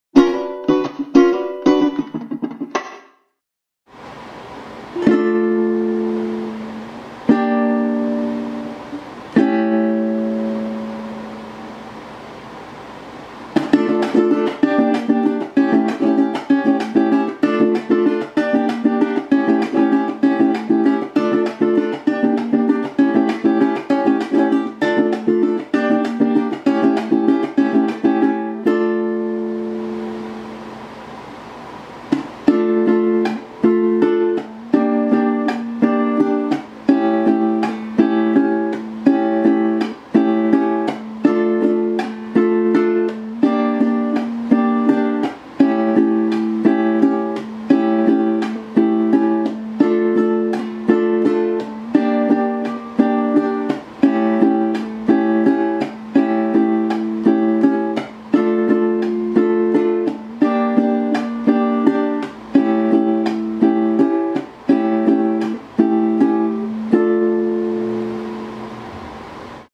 Cuatro Venezolano - Cómo tocar Joropo Pajarillo en Re menor Básico
Este es un ejemplo de cómo se toca el género del pajarillo con el cuatro venezolano en la tonalidad de Re menor. La primera parte del video muestra el Pajarillo a una velocidad estándar, un poco más lenta que de costumbre, donde se marca el primer golpe del compás en silencio (o con un frenado muy leve hacia abajo). La segunda parte del video muestra el mismo ritmo pero a una velocidad bastante lenta, casi a la misma velocidad de una tonada, para que puedas observar con detenimiento como se hace el ritmo.

Los acordes a tocar son Re menor, Sol menor y La Septima.